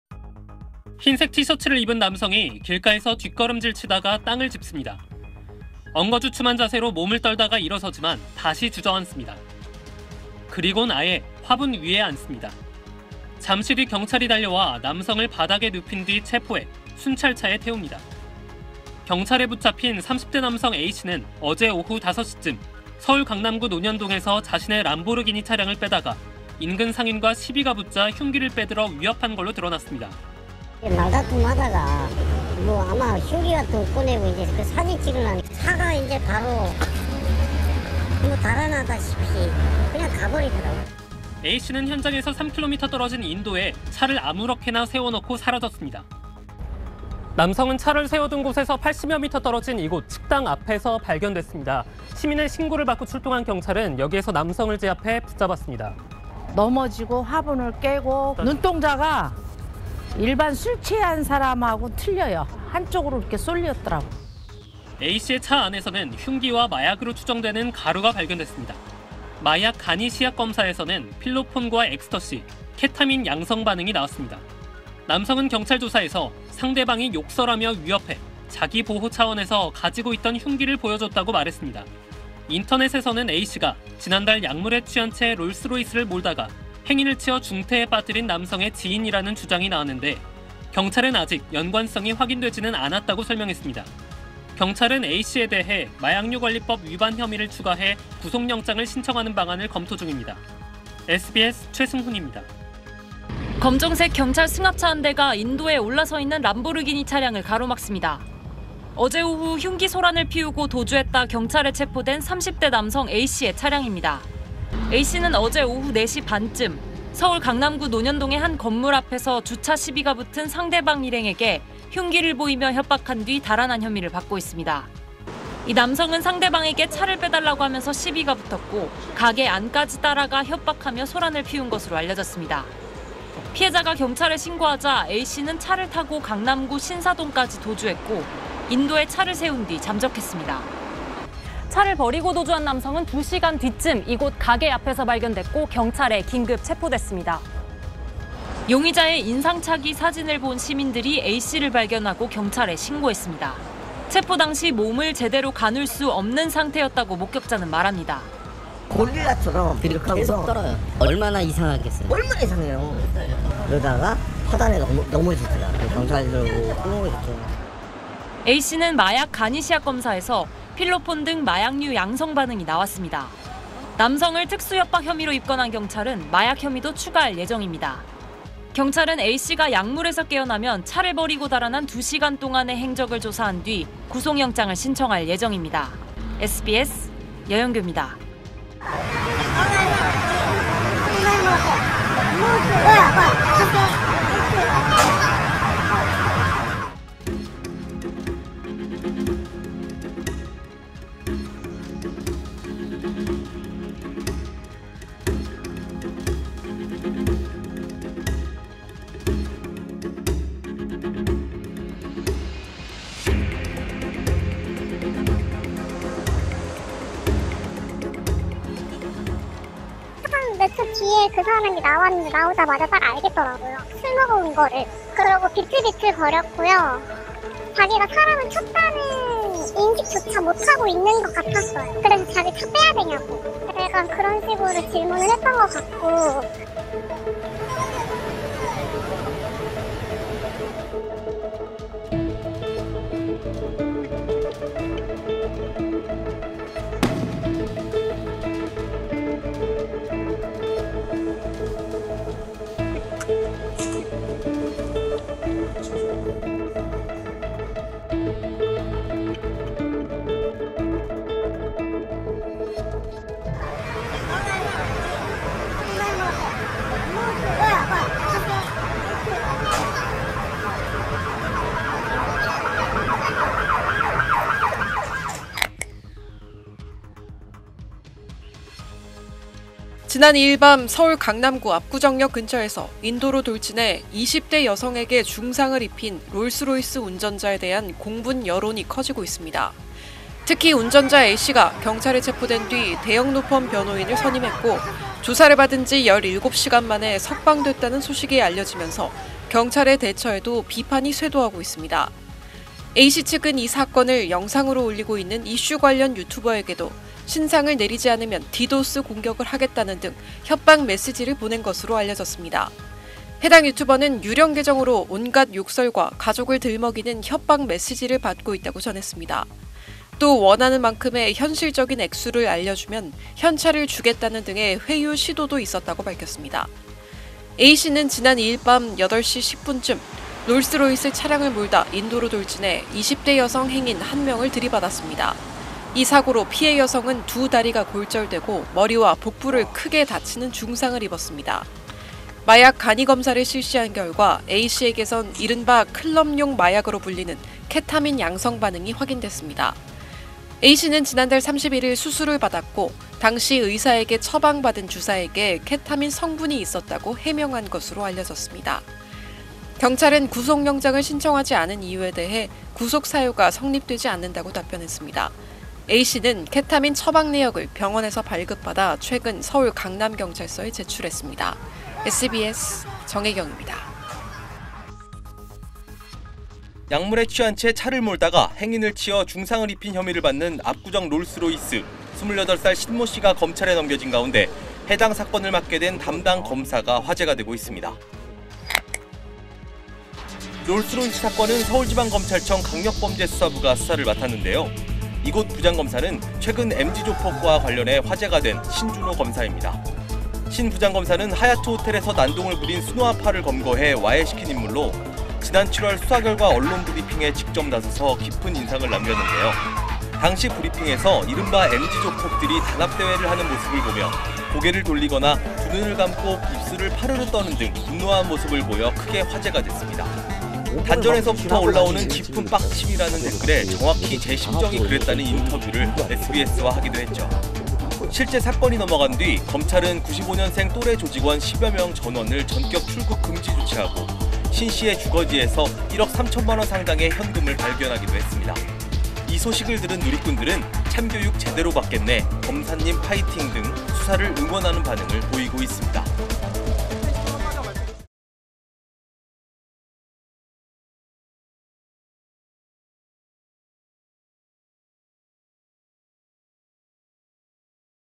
롤스로이스·람보르기니·벤틀리…마약 취해 난동 또 난동 / SBS / 모아보는 뉴스
00:00 "눈동자가 달라요" 부들부들 떠는 몸…체포 당시 '이상한 몸짓'
01:43 "고릴라처럼 계속 떨어요"…흉기 난동 부린 남성, 마약 양성
03:09 01:26 사람 들이받고 비틀거리며 '횡설수설'…음주운전인 줄 알았는데 '마약 운전'?
04:39 압구정 롤스로이스 운전자 석방 논란…유튜버 협박도
06:15  "롤스로이스남 제대로 망했네"…모두 놀란 담당 검사의 정체